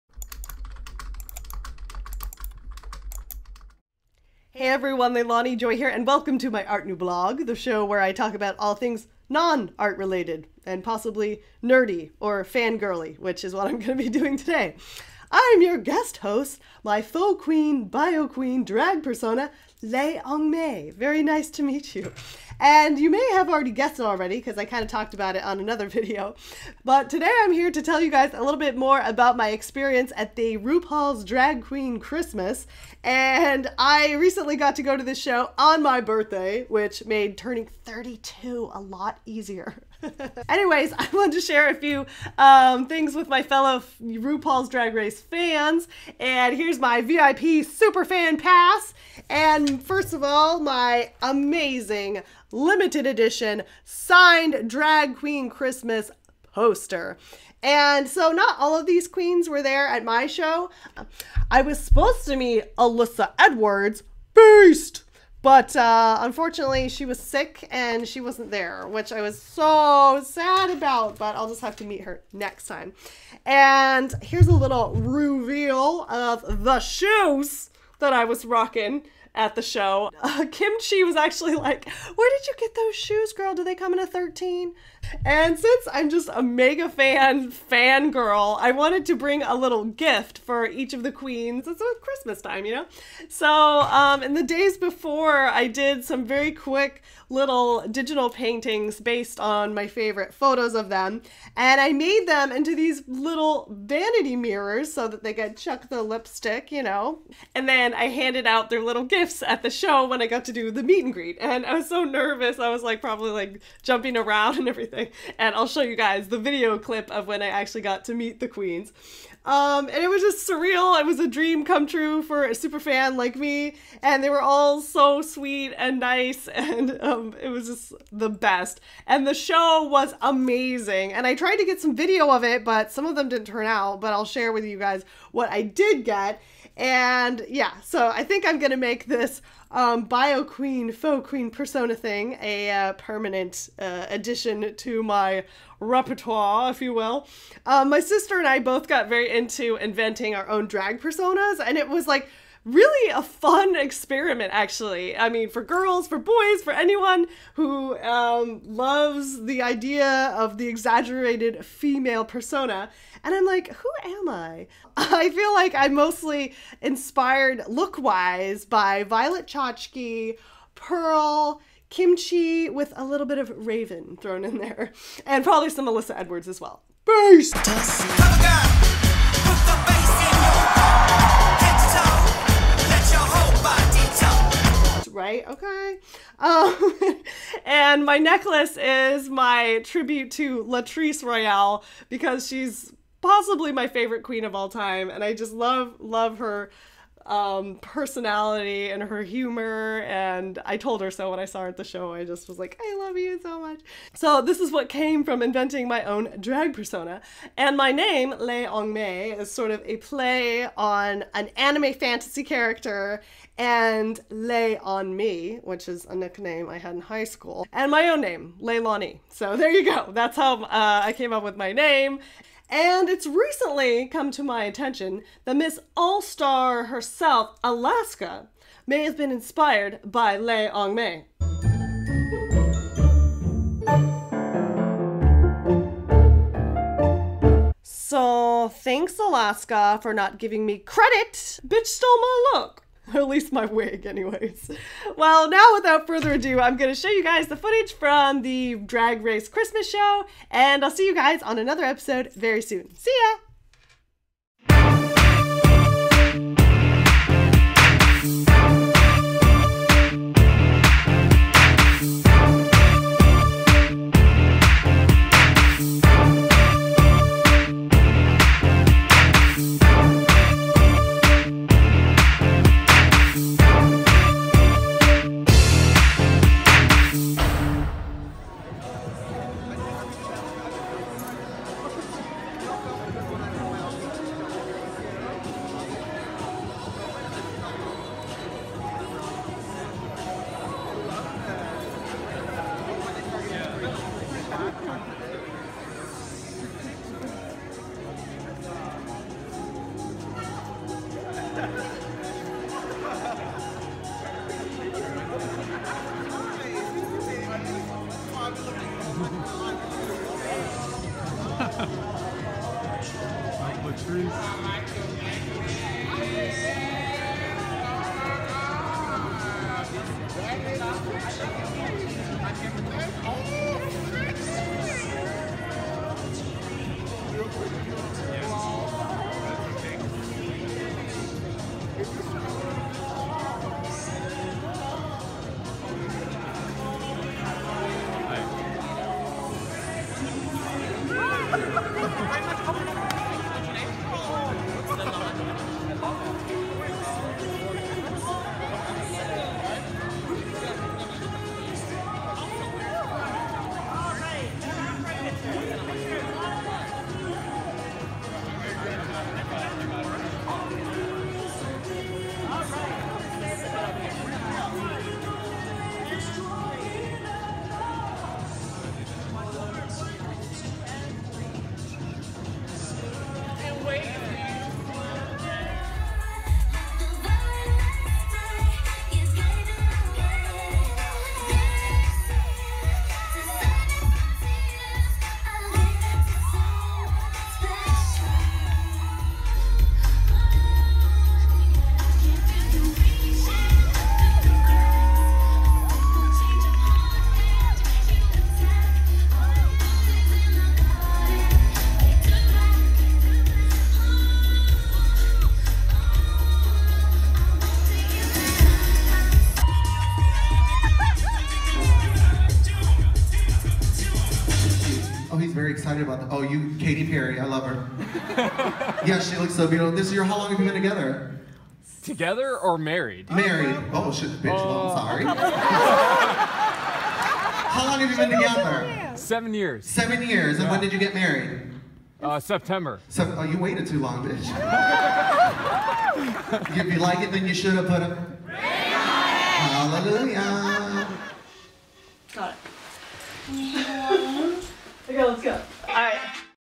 Art NuBlog: RuPaul's Drag Race Christmas Show 2016
As promised, a little ArtNuBlog from a DragRace Super Fan!
(re-uploaded so it should be viewable by all now!)